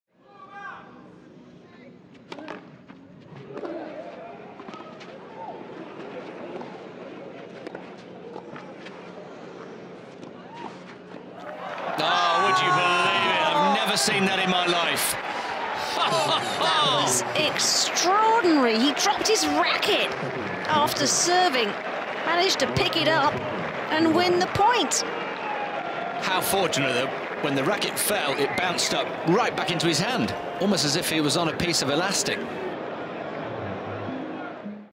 Fabio Fognini Drops Racquet, Still Wins Point | Rio Open 2018
Fabio gonna Fabio!